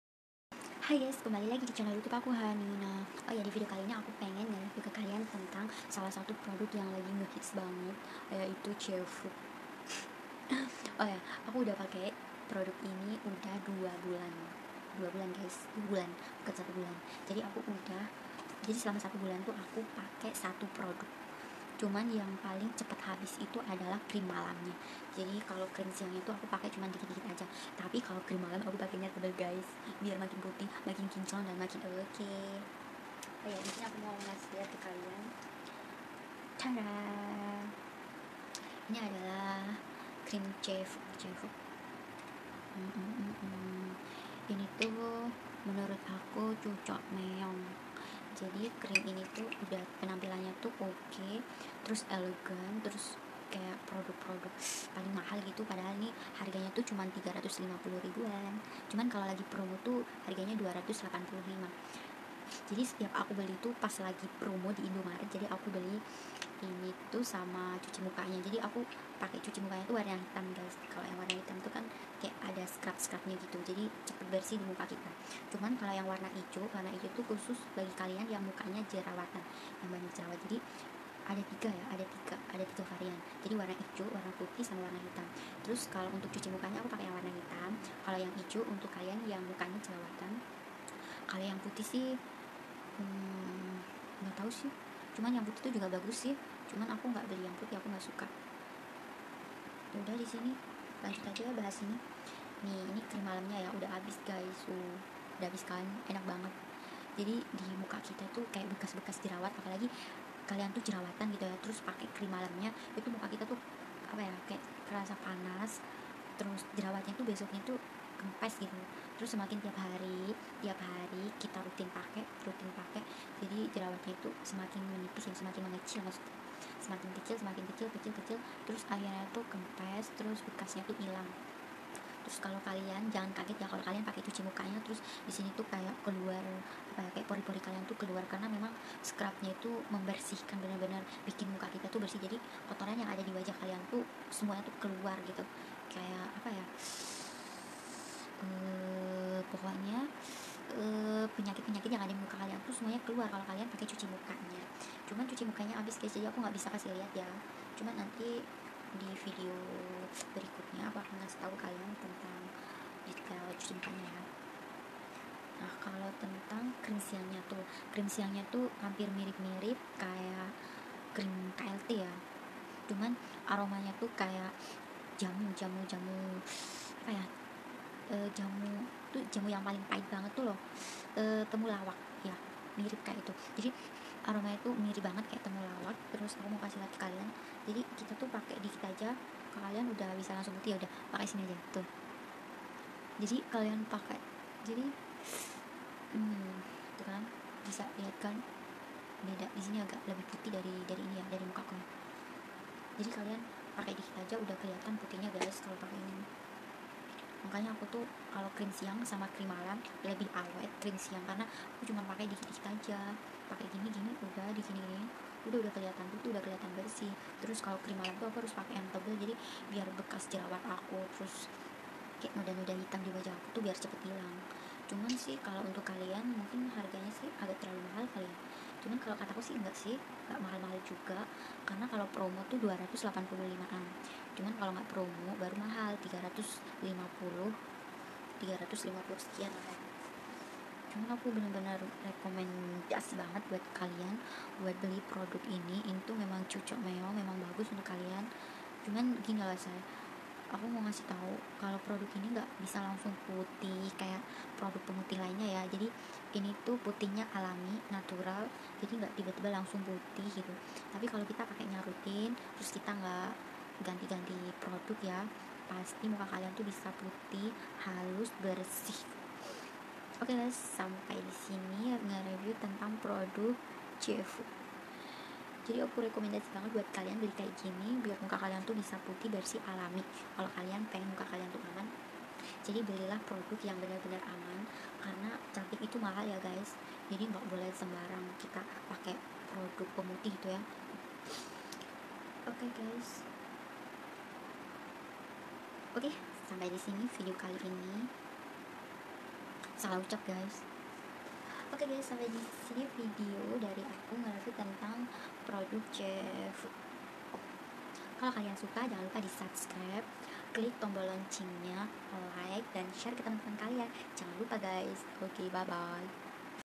Review TJE FUK Waouu BIKIN GLOWING#viral#vlogger#trending#viral#fashion
Assalamualaikum kaum hawa??
Usahakan nonton Ampe habis guys biar GK salah faham 😔 apapun yang kalian pake selama kalian rutin dan telaten hasil bisa memuaskan,ketika memakai skincare mahal ataupun murah tapi cocok di muka yang penting Aman teruskan aja ya biar gak rusak muka kalian!!
bantu support Chanel YouTube Ade aku👉 Hasan Santri Nusantara ✊🙏
👉 official Nina 🤗🙏